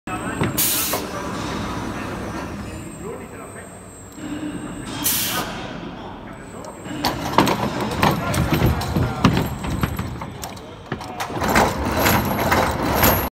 Filter press mod. FB2000x2000
Mining sector (soda). Filter press with membrane of Lencer, cake blowing, core blow system, washing with acid, high washing system of cloths, Warman pump with power 132 kw, all components in contacts with slurry are in steel AISI 316